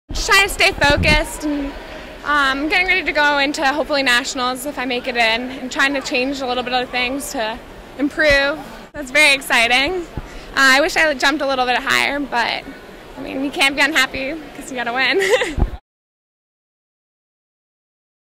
2014 A-10 Women's Pole Vault Champion - Mandissa Marshall, George Mason
George Mason senior Mandissa Marshall won the pole vault at the 2014 Atlantic 10 Track & Field Championship with a height of 3.90m